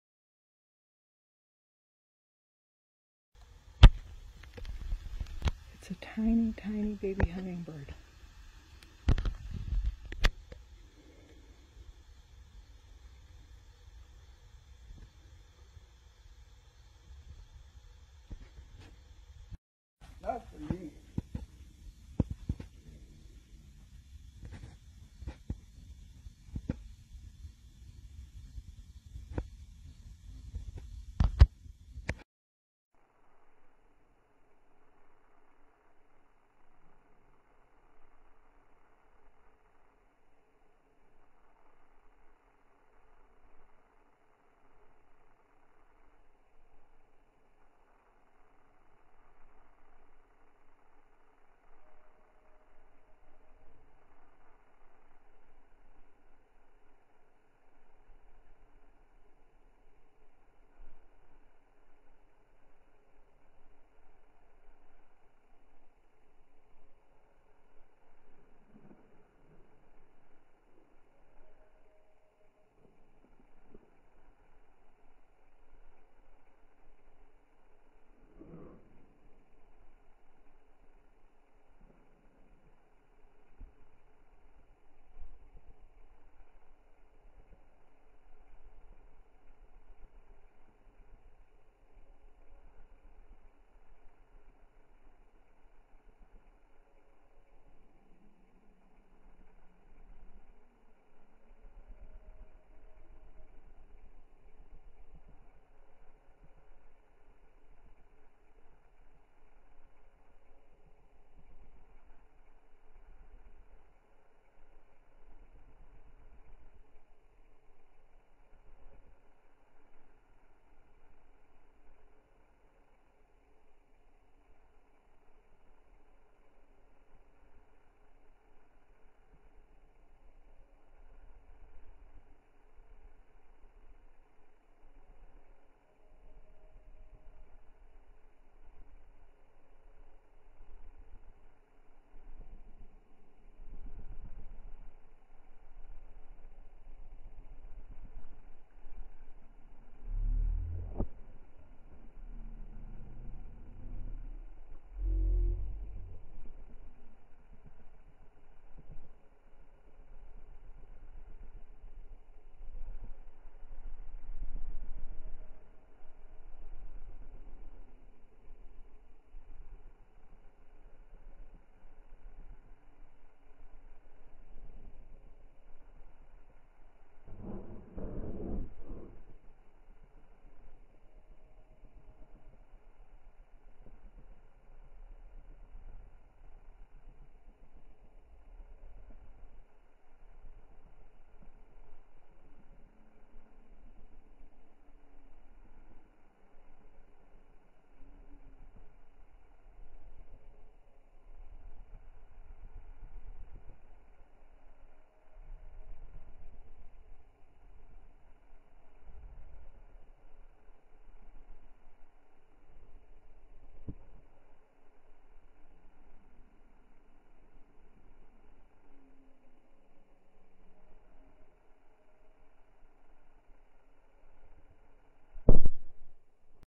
Baby Hummingbirds. The last part is in slow motion so you can really see them!
This is something I've never seen before. Baby hummingbirds! I saw 2. I believe the nest was inside of the lantana bush.